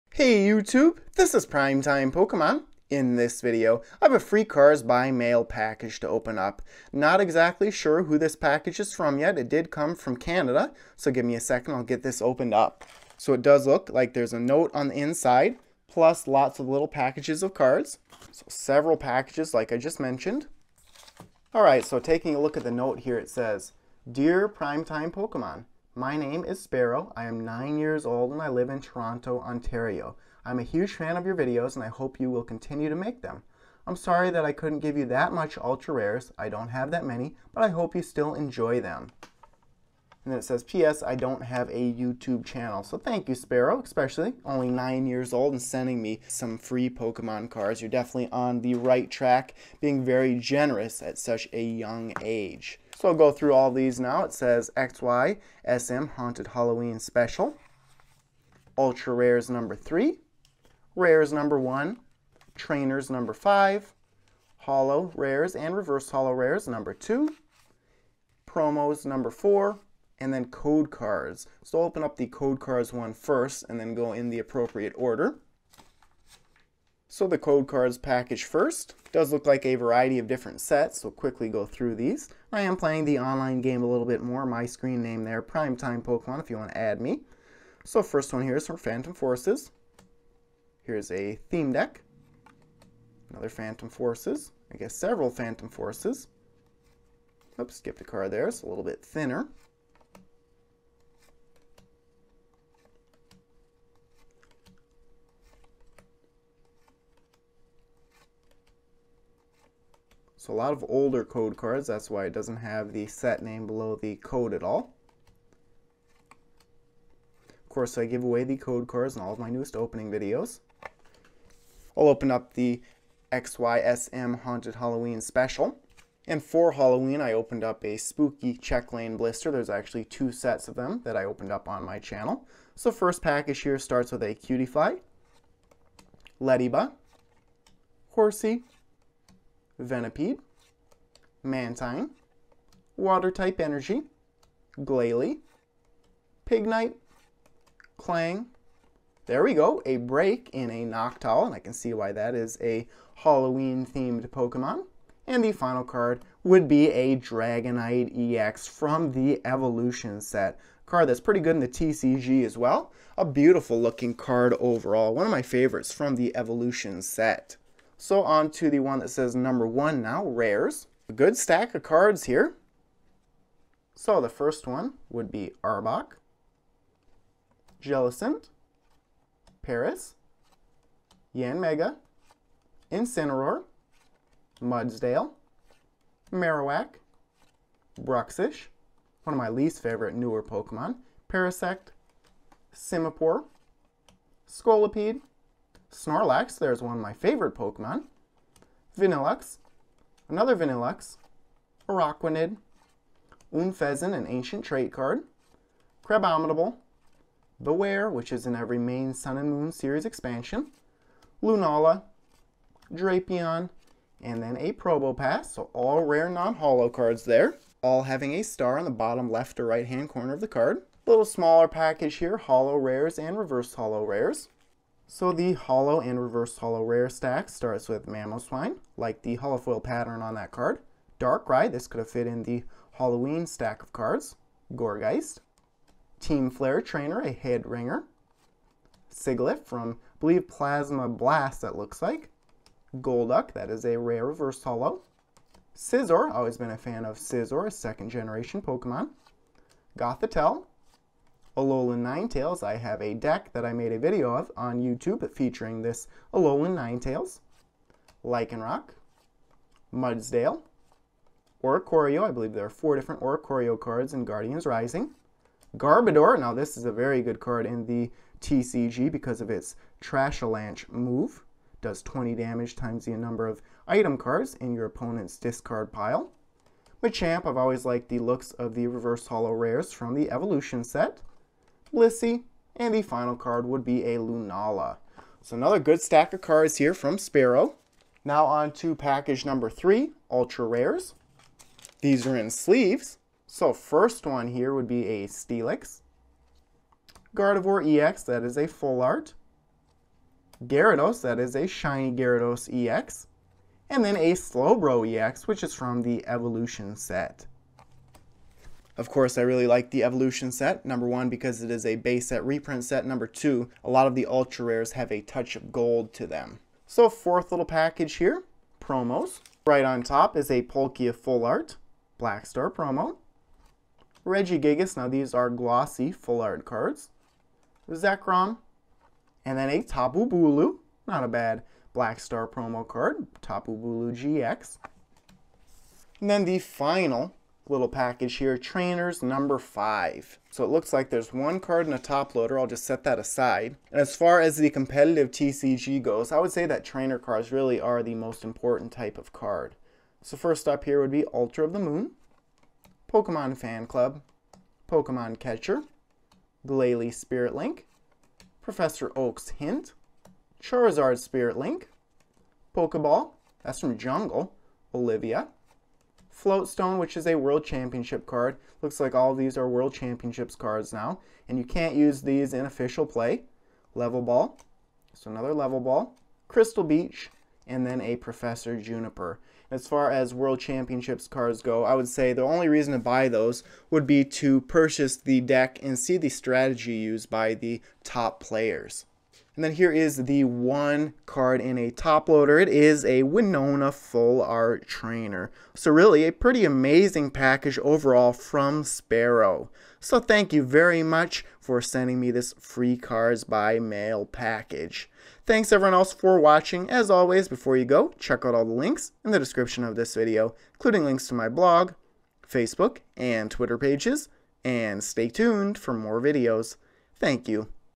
Free Pokemon Cards by Mail: Sparrow
A Free Pokemon Cards by Mail package from Sparrow is opened in this video. This 9 year old fan sent me a ton of great cards, including GX, EX, and Black Star Promo cards! I would like to thank Sparrow very much for the fan mail package!